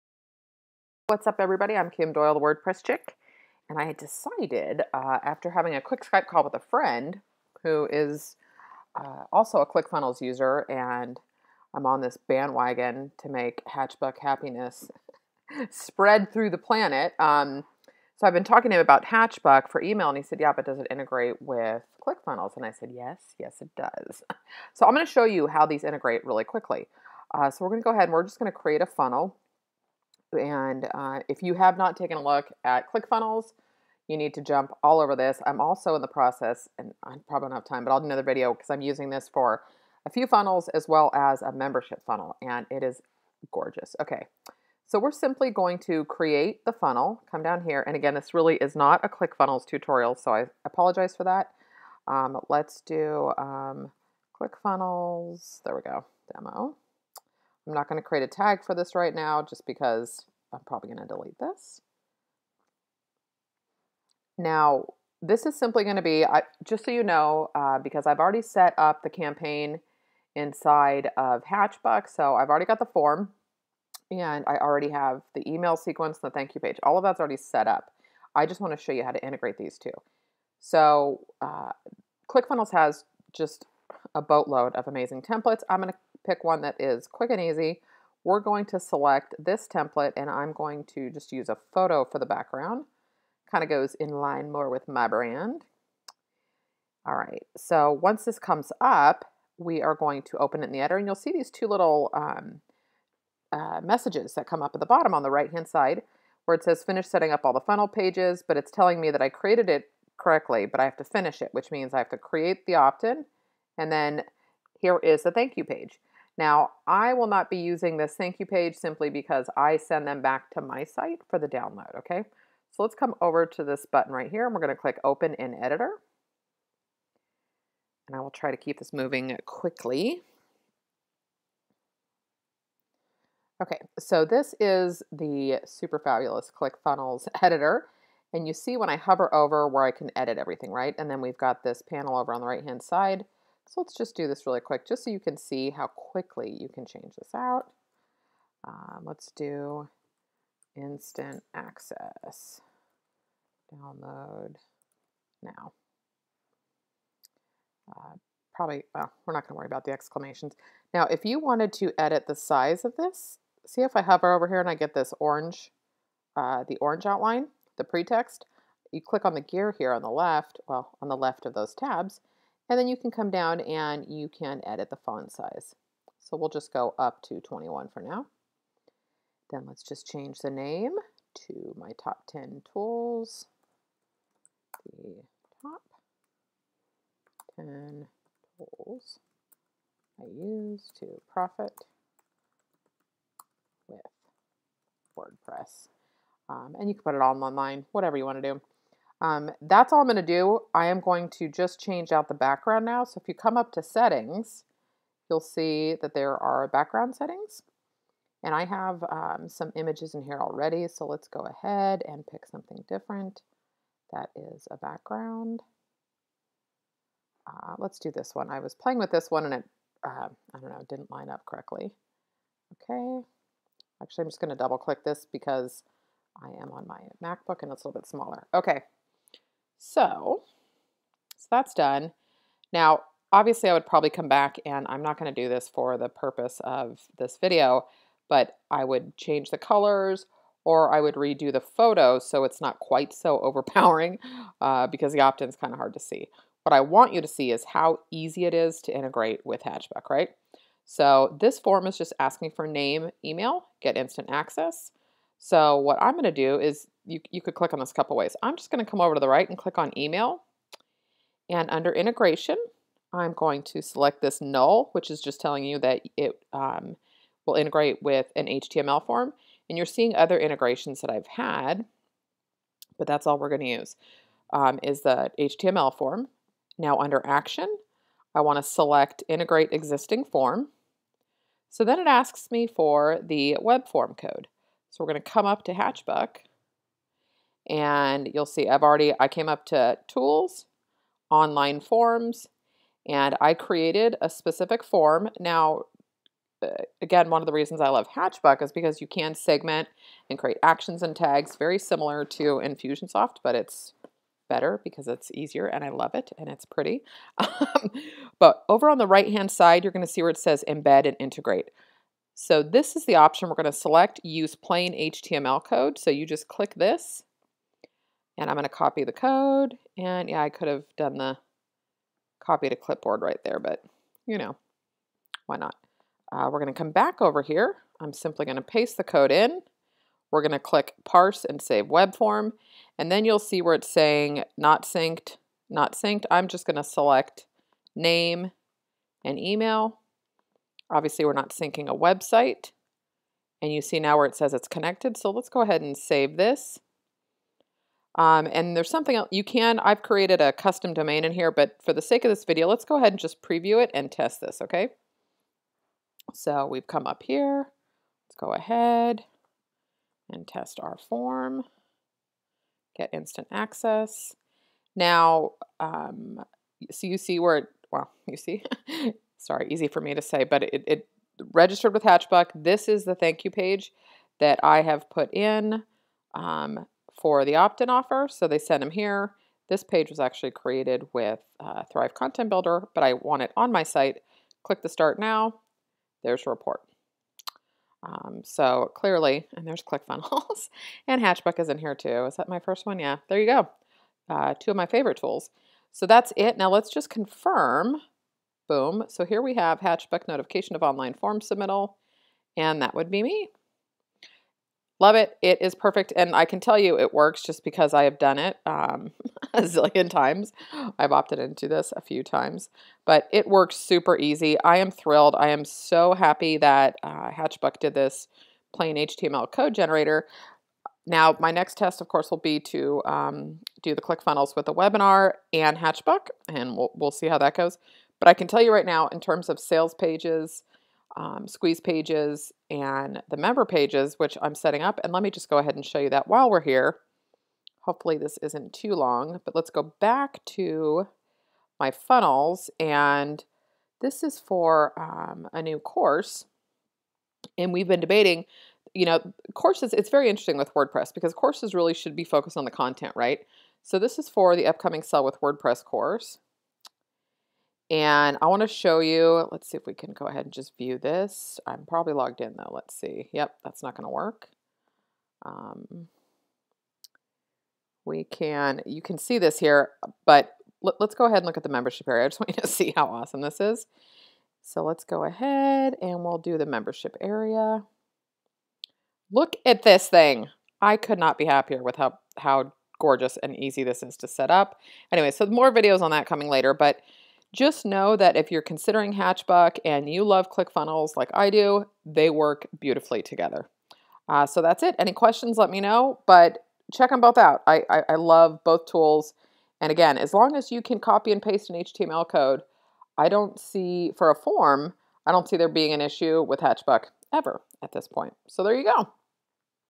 Hatchbuck ClickFunnels Integration
If you're a Click funnels user and have been thinking about making the move to Hatchbuck (goodbye Infusionsoft confusion), check out how easy it is to integrate Click funnels and Hatchbuck. More HatchbuckHappiness for this chick!